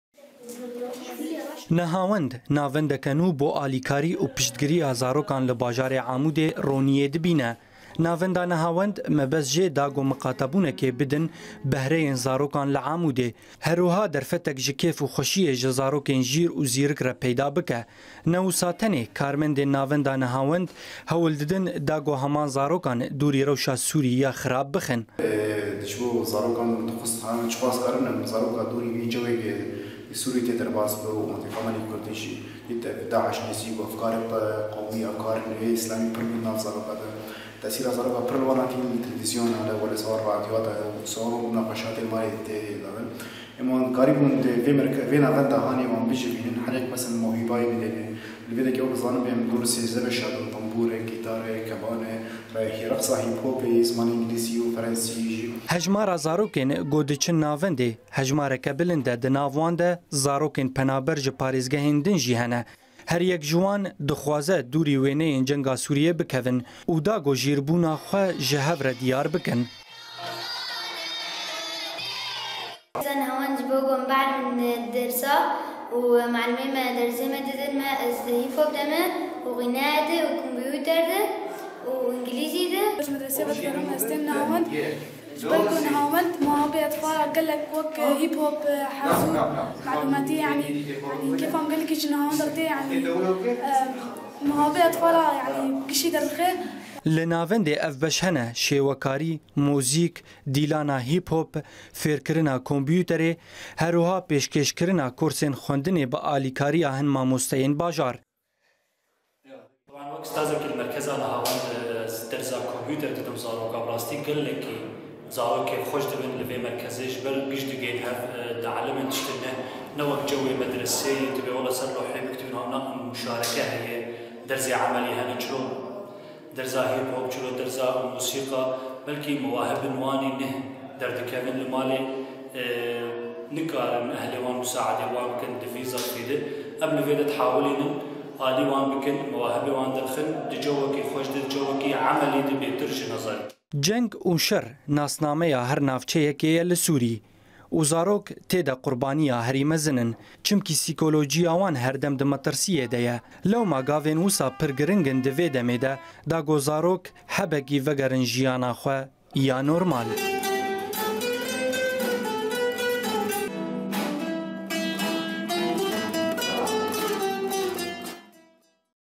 Amûdê Nehawend navendeke nû bû zarokên bajar e
Ji ber ku sîkolociya zarokan di dema cenga Sûriyê de herdem di rewşeke xirab de bû , lewma hin navend û rêxistinên miqatebûna zarokan li her bajarekî Rojavayê Kurdistanê avabûn , mîna navenda Nehawend li bajarê Amûdê